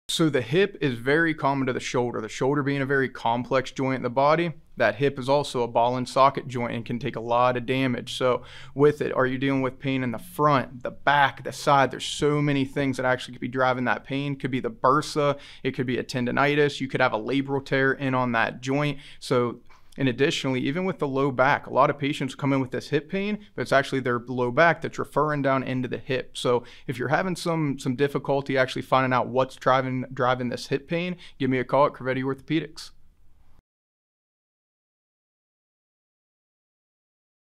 Crovetti Chiropractic - Hip Pain
The hip is very common to the shoulder. The shoulder being a very complex joint in the body, that hip is also a ball and socket joint and can take a lot of damage. So, with it, are you dealing with pain front, the back, the side, there’s so many things that could actually be driving that pain. It could be bursa, it could be tendonitis, you could have a labral tear in on that joint. So, in addition with the low back, a lot of patients come in with this hip pain, but it’s actually their low back that’s referring down into the hip. So, if you’re having some difficulty actually finding out what’s driving that hip pain, give me a call at Crovetti Orthopedics.